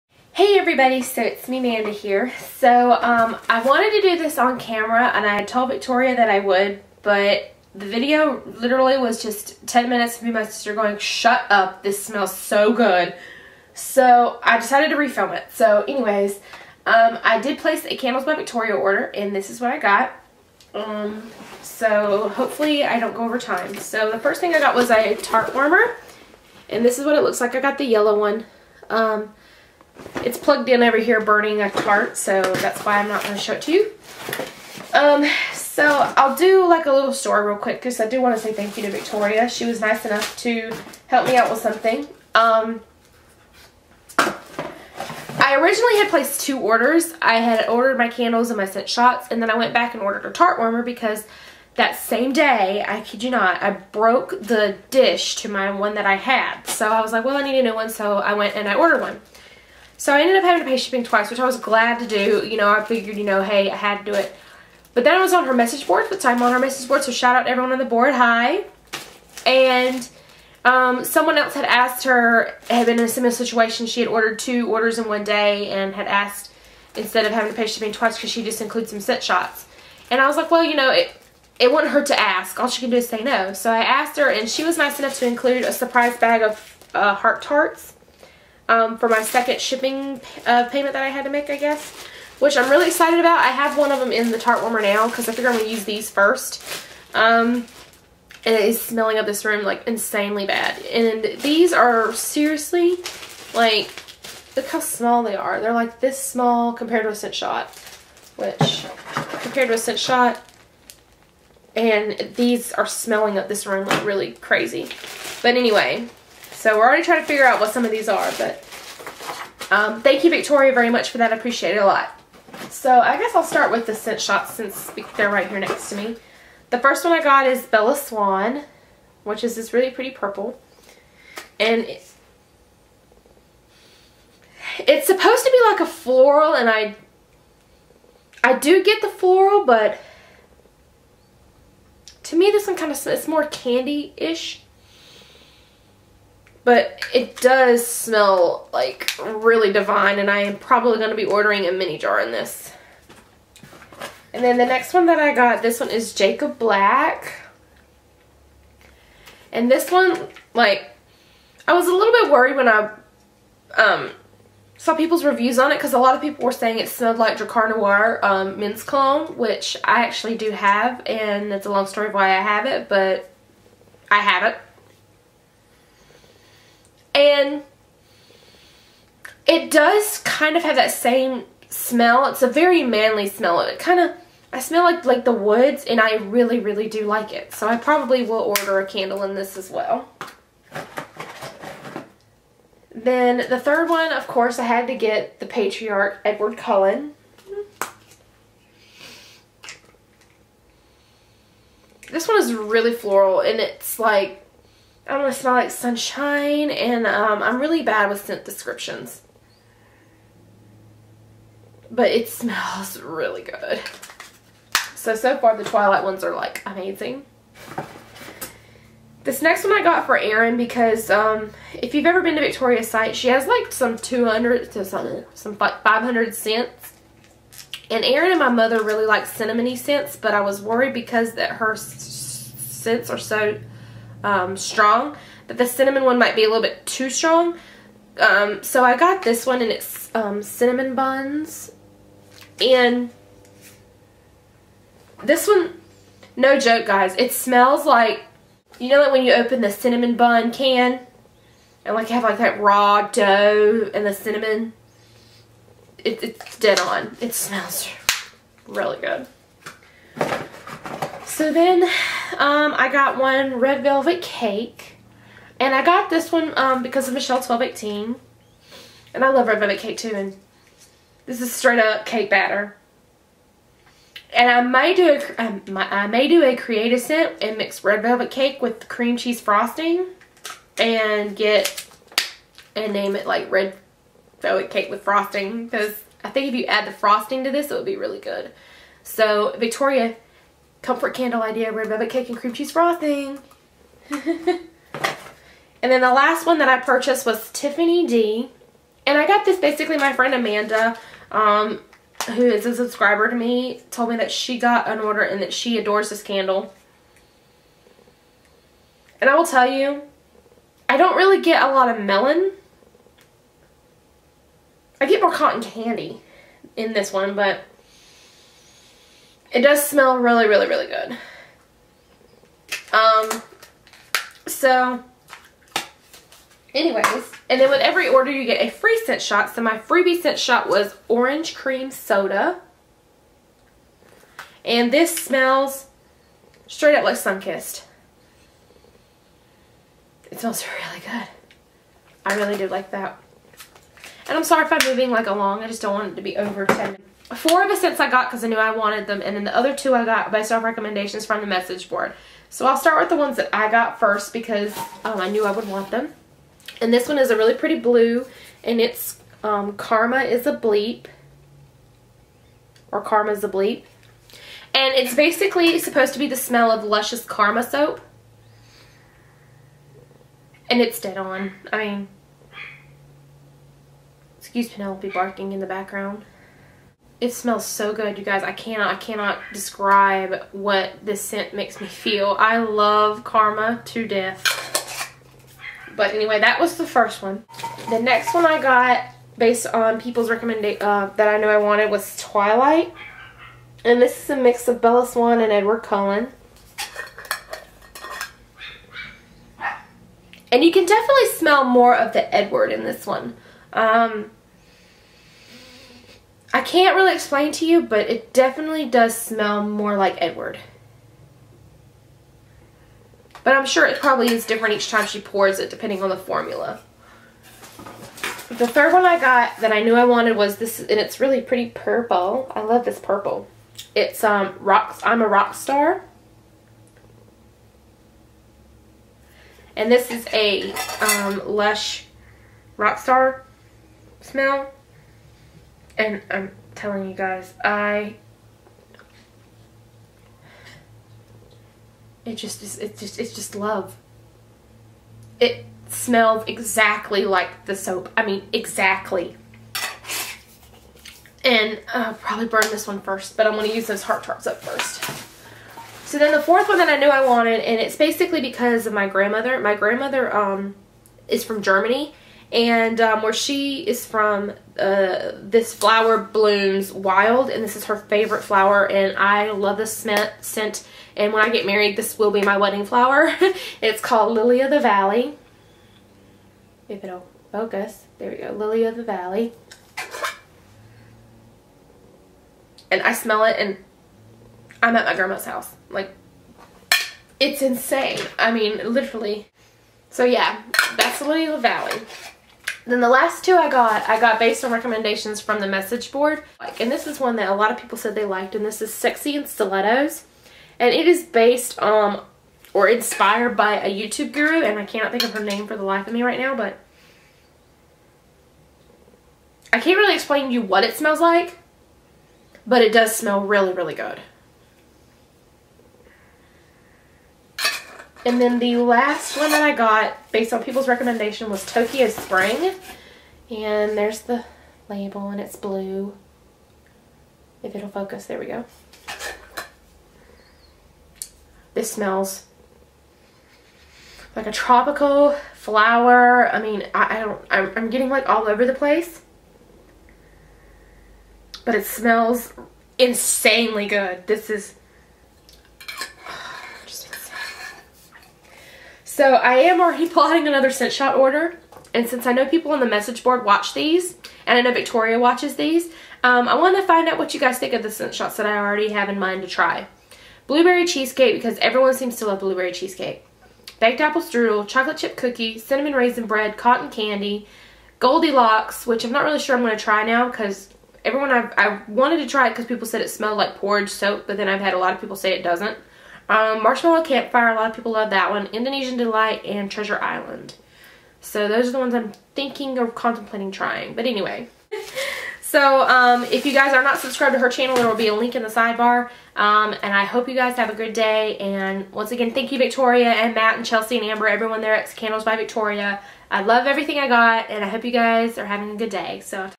Candles By Victoria
My first Candles By Victoria order!  All in all the shipping was fast and everything was packaged safely and securely.

Items Purchased

Bright Yellow Tart Warmer

SS:
Edward Cullen
Bella Swan
Jacob Black
Red Velvet Cake
Tiffany-D
Cinnamon Buns

SS FREEBIE - Orange Cream Soda

8oz. Candle Sampler pack -

I'm a Rockstar
Karma's a Bleep
Lily of the Valley
Twilight
Tokyo Springs
Sexy in Stilettos